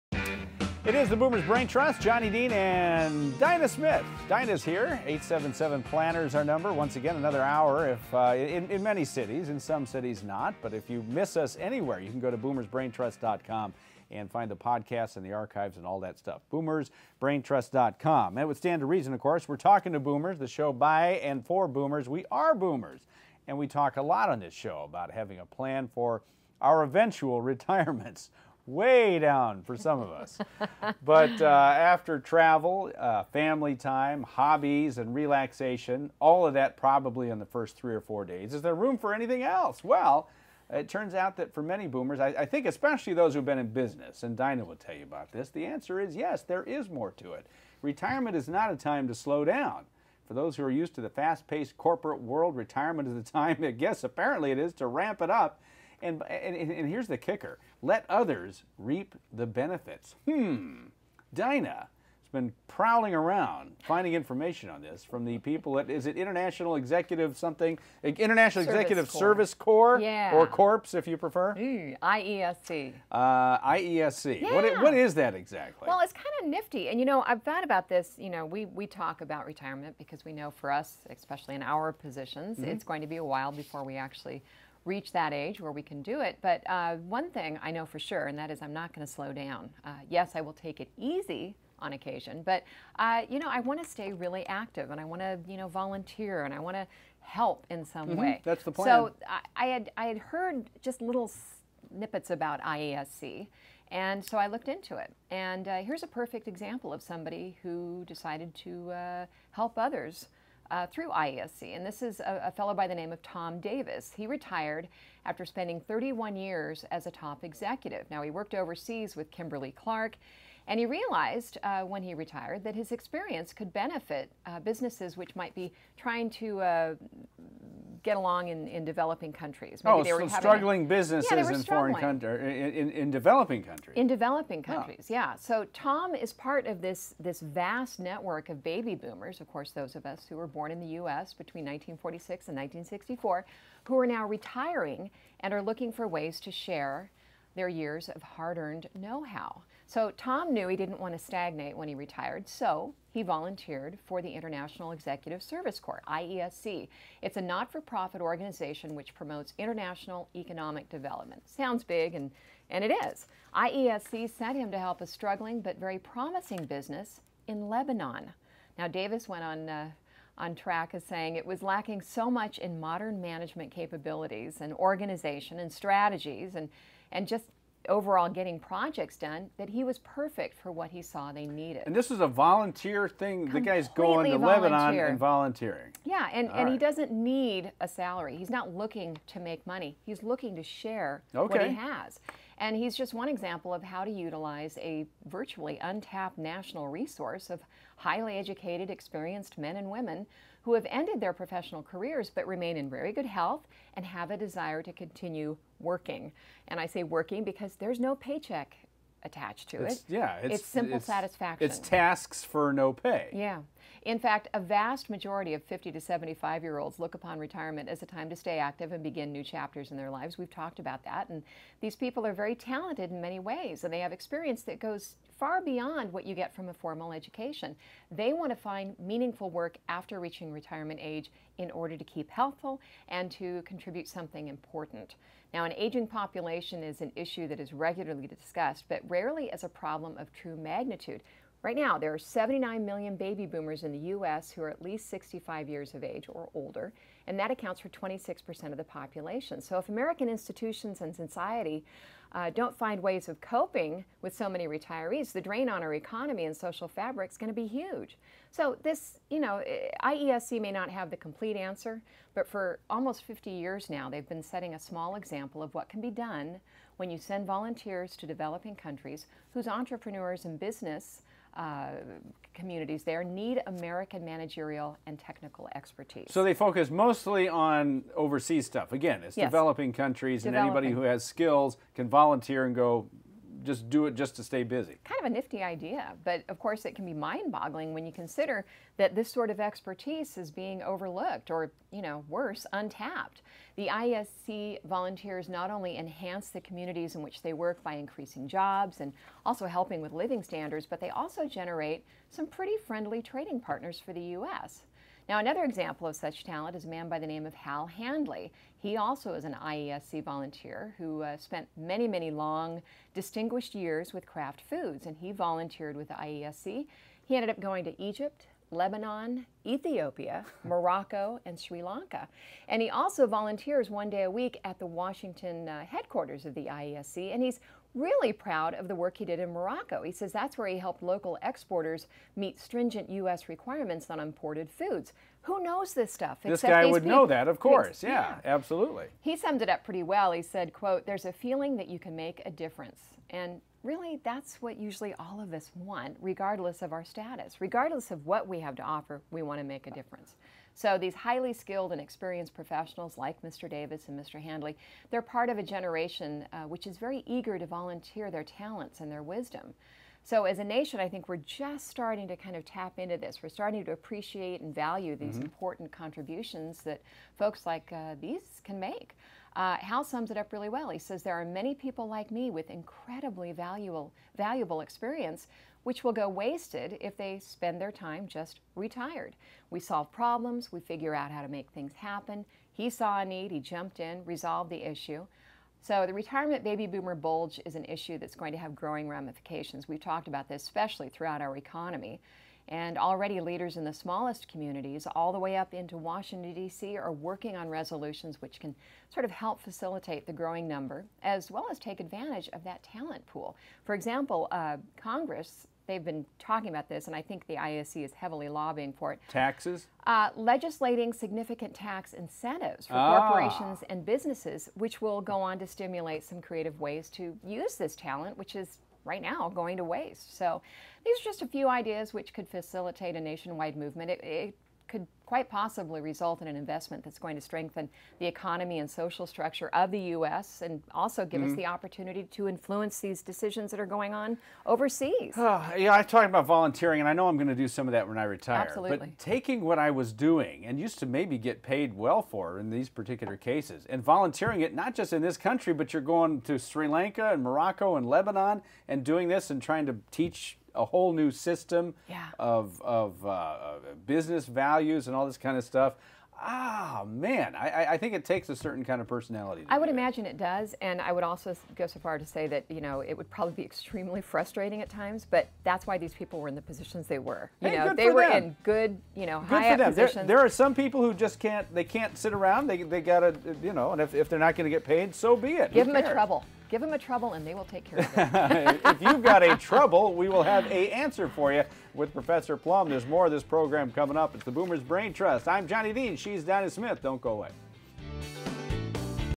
Volunteering: the untapped value of boomers
Boomers who are now retiring and looking for ways to share their years of hard earned know-how are volunteering for the International Executive Service Corps.